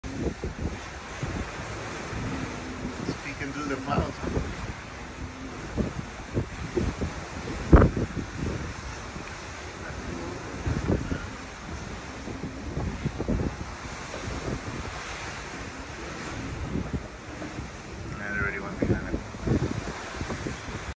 Sunrise On The Way To Puerto Rico Onboard The NCL Breakaway Part 2
Love waking up early to watch the sunrise!